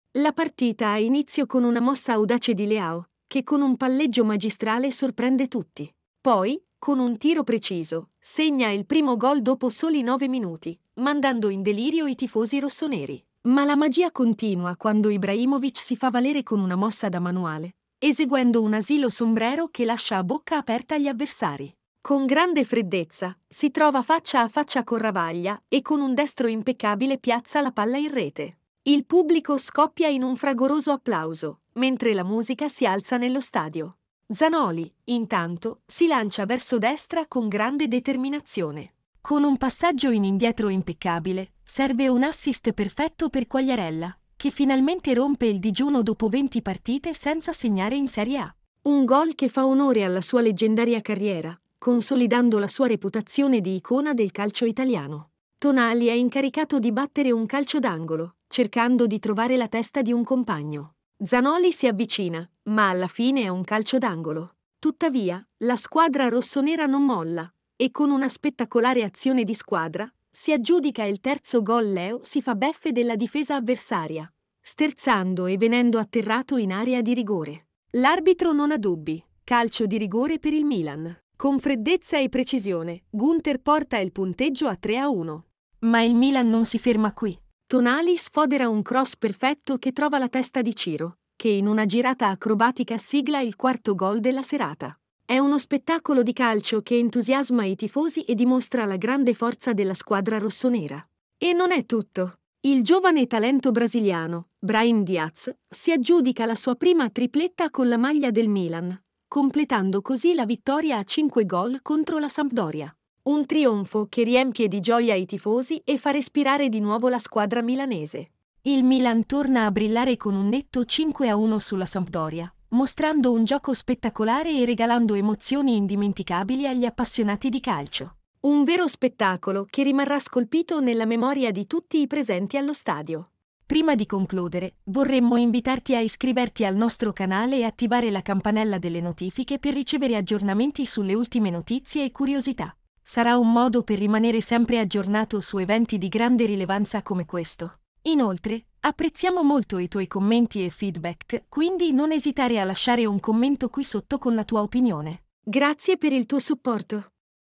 unesclusiva per gli appassionati di calcio il milan fa incetta di gol nella spettacolare vittoria
Benvenuti nel video "Un'esclusiva per gli appassionati di calcio: il Milan fa incetta di gol nella spettacolare vittoria contro la Sampdoria!" In questa partita emozionante, la squadra del Milan ha dimostrato tutto il suo talento e la sua determinazione, segnando gol a raffica. I tifosi sono stati trattati con una performance calcistica spettacolare, mentre il Milan si è assicurato una vittoria trionfale. Ibrahimovic, Leão, Quagliarella, Tonali e molti altri hanno dimostrato la loro magia sul campo, con palleggi incredibili e gol indimenticabili. Questo video è un'emozionante esclusiva per gli appassionati di calcio, che potranno rivivere ogni istante di questa serata di follia calcistica. Un mix di emozioni, sport e una squadra in grande forma che conquista il campo. Non perdete questa incredibile partita che ha regalato emozioni uniche e una vittoria spettacolare al Milan.